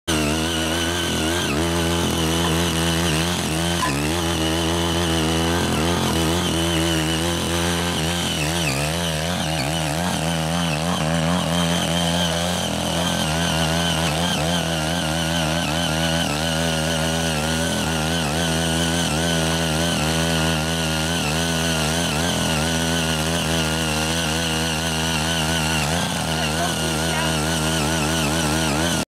混雜土質鬆土CY80 Power tiller , tilling in the miscellaneous soil-2 擎億機械 농업 기계 中耕機
混雜土質鬆土CY80 Power tiller , tilling in the miscellaneous soil-2 擎億機械

For Tea ,Bamboo , Vegetables ...etc Plantation / Orchard / Dry Paddy Field / Flora Garden / Home Garden / Greenhouse

Using area : all kinds of landform with Hard soil (black, red) / Narrow area / Hilly area / Broken Earth area / Gravel Area / Moist , Soft soil